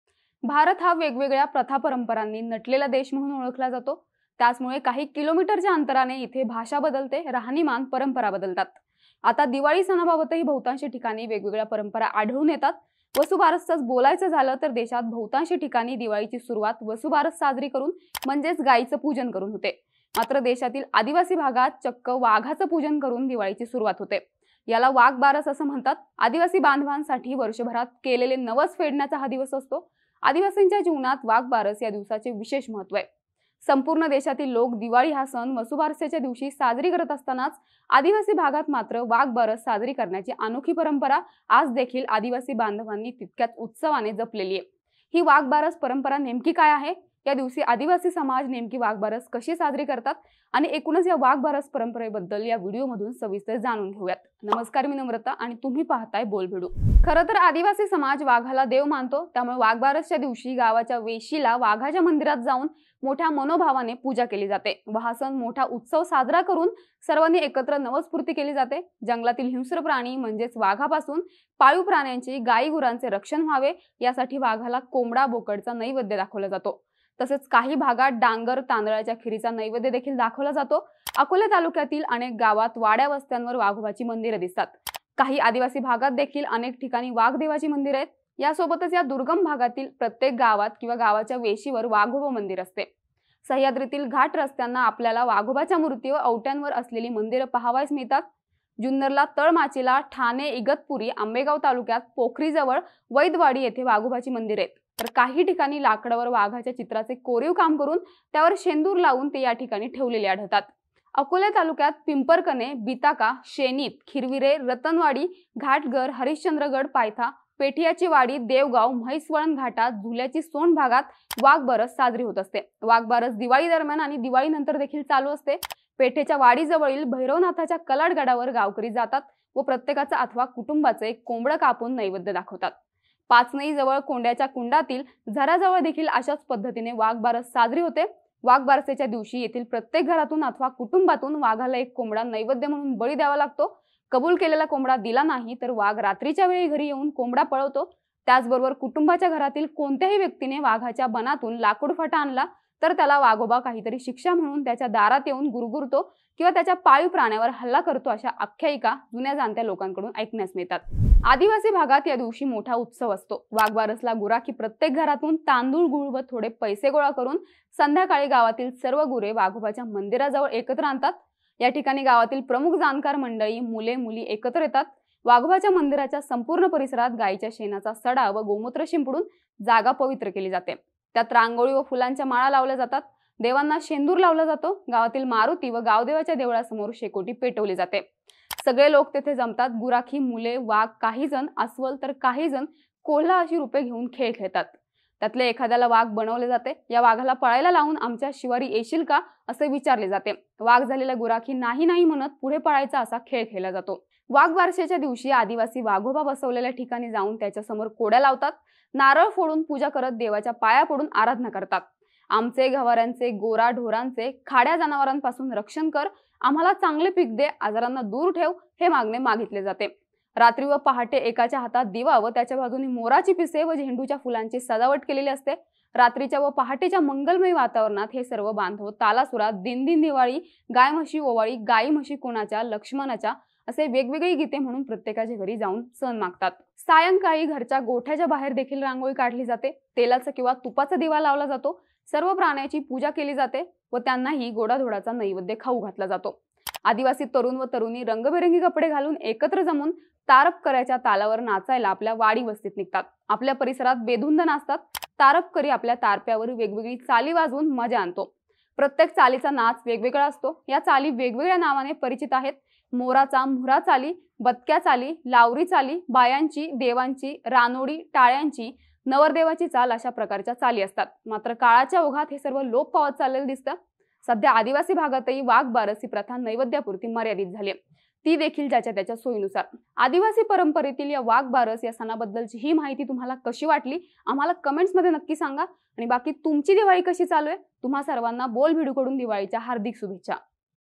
Diwali ला Vasubaras साजरी न करता आदिवासी बांधव Waghbaras साजरी करतात. अशी आहे Waghbaras ची परंपरा.
भारत हा वेगवेगळ्या प्रथा परंपरांनी नटलेला देश म्हणून ओळखला जातो. त्याचमुळे काही किलोमीटरच्या अतंराने इथे भाषा बदलते, राहणीमाण, परंपरा बदलतात. आता दिवाळी सणाबाबातही बहुतांश ठिकाणी वेगवेगळ्या परंपरा आढळून येतात. वसूबारसचच बोलायचं झालं तर देशात बहुतांश ठिकाणी दिवाळीची सुरुवात वसूबारस साजरी करून म्हणजेच गायीचं पुजन करून होते. मात्र देशातील आदिवासी भागात चक्क वाघाचं पुजन करून दिवाळीची सुरुवात होते. याला वाघबारस असं म्हणतात.

 संपूर्ण देशातील लोक दिवाळी हा सण वसुबारसेच्या दिवशी करत असतानाच अदिवासी भागात मात्र वाघबारस साजरी करण्याची अनोखी परंपरा आजही आदिवासी बांधवांनी  तितक्याच उत्सवाने जपली आहे.  ही वाघबरस परंपरा नेमकी काय आहे  या दिवशी आदिवासी समाज नेमकी वाघबारस कशी साजरा करतो आणि एकूणच या वाघबारस परंपरेबद्दल या व्हिडीओतून सविस्तर जाणून घेवूयात.